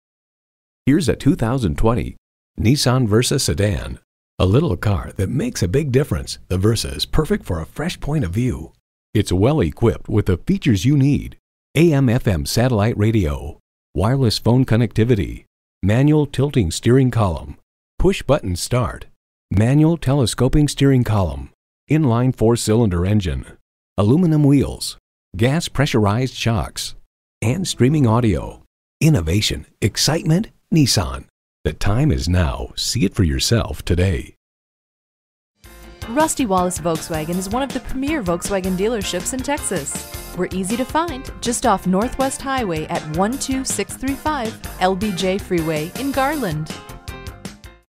Used 2020 Nissan Versa Dallas TX Garland, TX RRP9657 - SOLD
Year: 2020
Make: Nissan
Model: Versa
Trim: 1.6 SV
Engine: 1.6L 4-Cylinder DOHC 16V
Transmission: CVT
Color: Black
Interior: Graphite
Mileage: 63447
Stock #: RRP9657
VIN: 3N1CN8EV8LL876018
Super Black Metallic 2020 Nissan Versa 1.6 SV FWD CVT 1.6L 4-Cylinder DOHC 16V BACK UP CAMERA, ALLOY WHEELS, Versa 1.6 SV, 1.6L 4-Cylinder DOHC 16V, Super Black Metallic.brbrRecent Arrival! 32/40 City/Highway MPGbrbrAwards:br * JD Power Automotive Performance, Execution and Layout (APEAL) Study

Address:
12635 Lyndon B Johnson Freeway